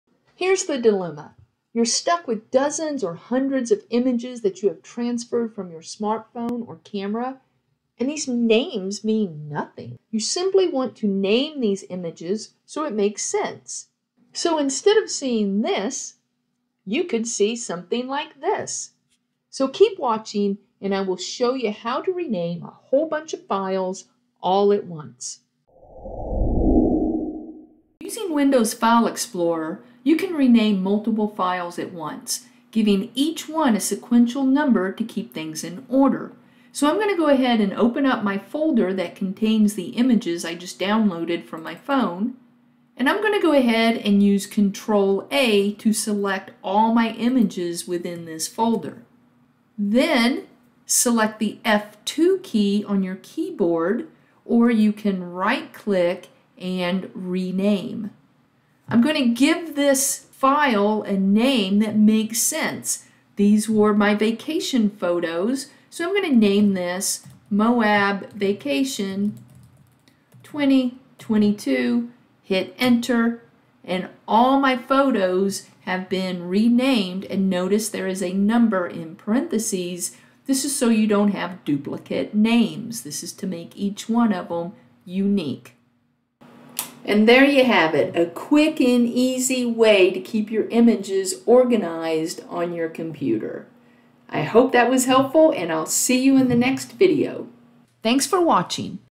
Quickly & Easily Rename Multiple Images
You just transferred some photos from your smartphone to the computer and you have all these file names like this IMG_20220617_11213580.jpg. Good grief!!
You can quickly rename all those files at once.

Note: This video is for Windows users. I am demonstrating in Windows 11, but this method works in all Windows versions.